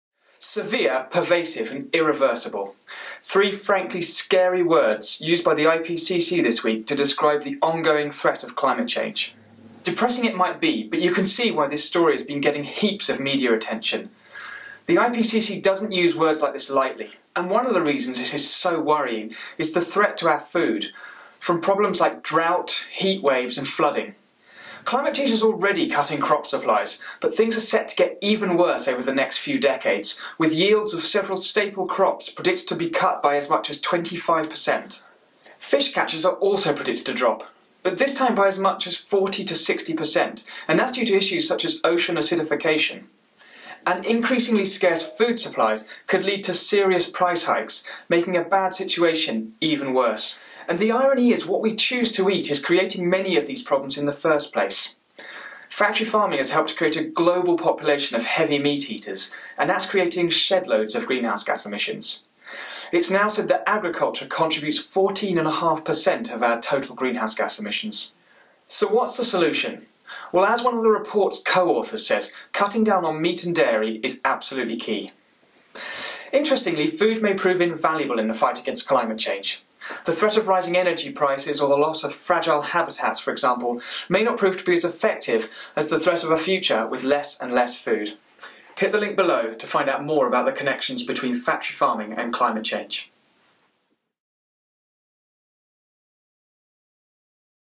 Boiling point: Climate change and the end of food as we know it?
Campaigner, Jonty, discusses the links between factory farming, climate change and food production. A hot topic given the recent release of the IPCC's latest report, which doesn't make for easy reading.